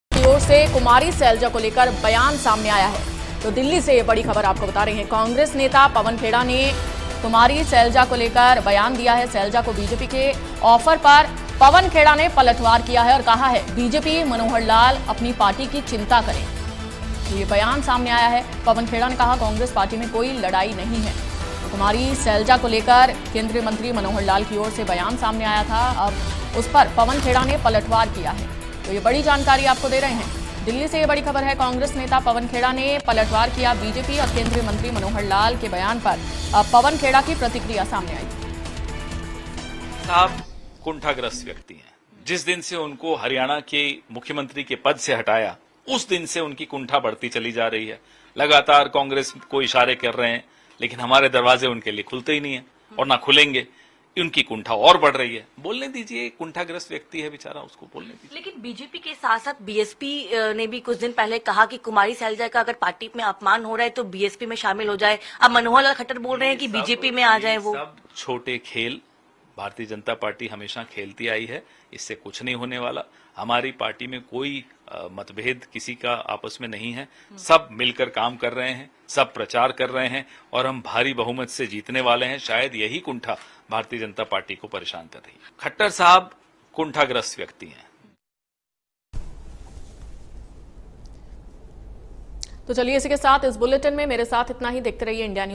Kumari Selja को BJP के ऑफर पर Pawan Khera का पलटवार, Manohar Lal ने ऐसे दिया जवाब ! | India News
Kumari Selja को BJP के ऑफर पर Pawan Khera का पलटवार, Manohar Lal ने ऐसे दिया जवाब ! | India News.. Selja के BJP Join करने की ख़बरों के बीच Congress Leader Pawan Khera ने बड़ा बयान दिया है. उन्होंने कहा है कि Congress एकजुट और एकसाथ है..